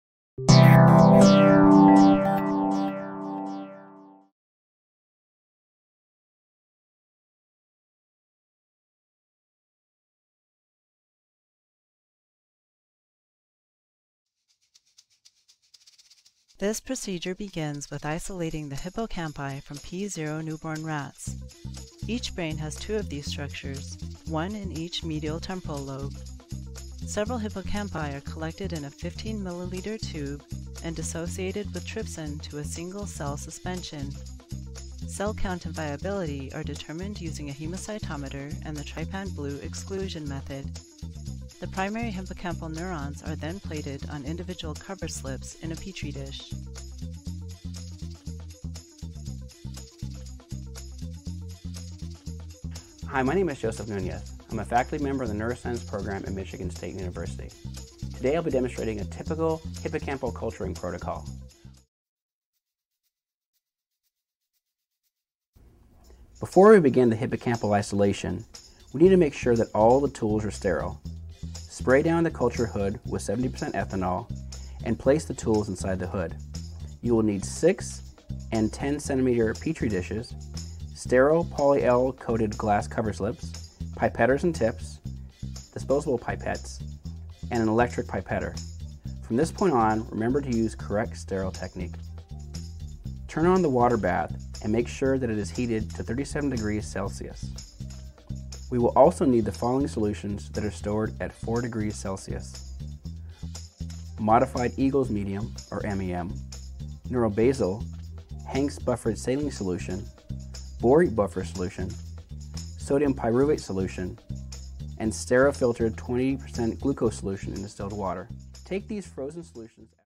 Primary Culture: Hippocampal Neurons From P0 Newborn Rats l Protocol Preview
Primary Culture of Hippocampal Neurons from P0 Newborn Rats -  a 2 minute Preview of the Experimental Protocol

Joseph Nunez
Michigan State University (MSU), Department of Psychology;

The dissection and growth of cells from an individual brain area facilitates investigation of cellular and physiological parameters. We describe a method for primary cell culturing that produces neuron-enriched cultures in a serum-free environment.